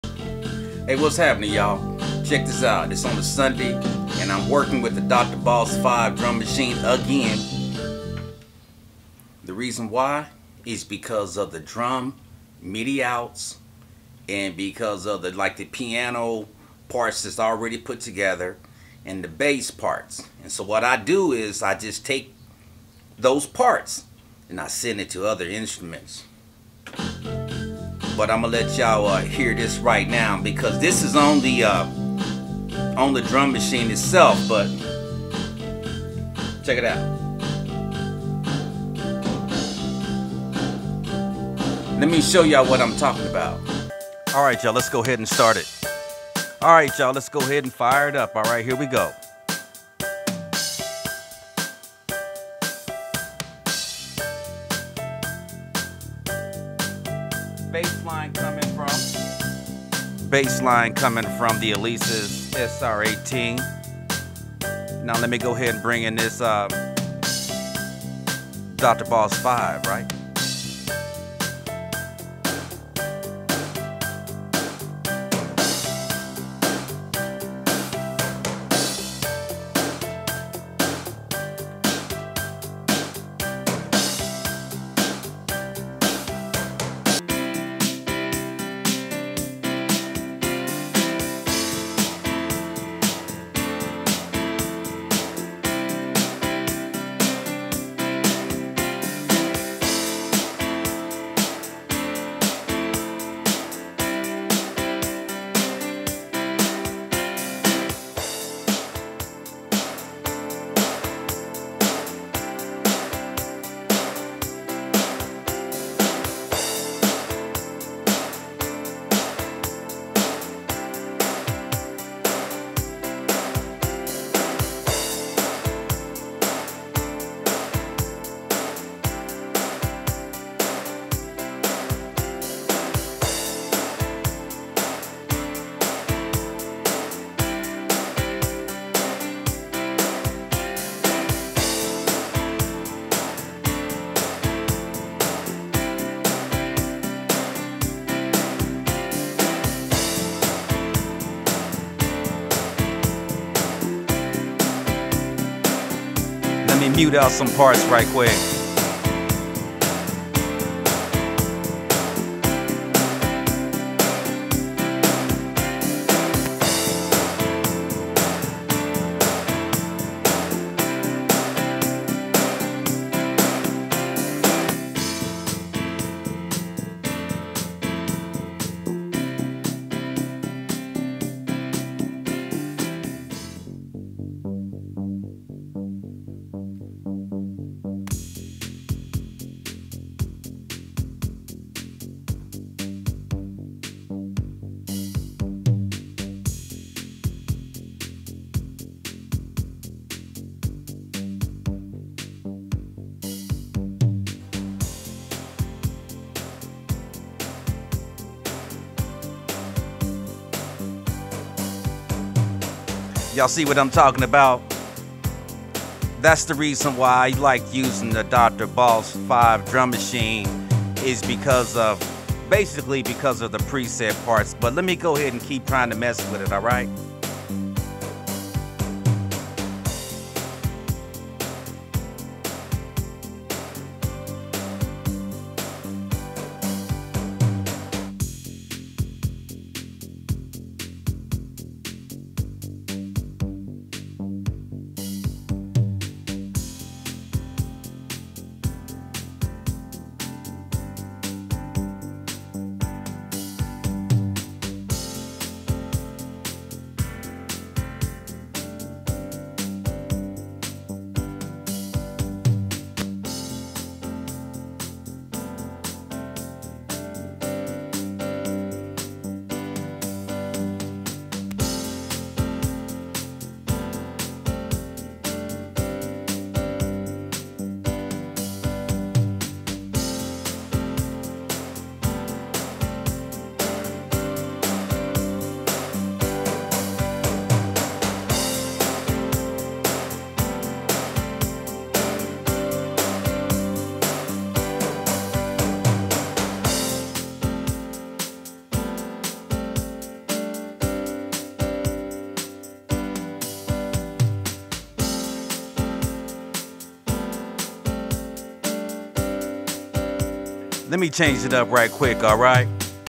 Why I Like the DR BOSS DR 5 presets
The BOSS DR-5 presets can be used. The Drum machine has Rock, Rock Ballads, Jazz, Soul. I like to use the midi features of the machine. Because I can layer my sounds over several instruments.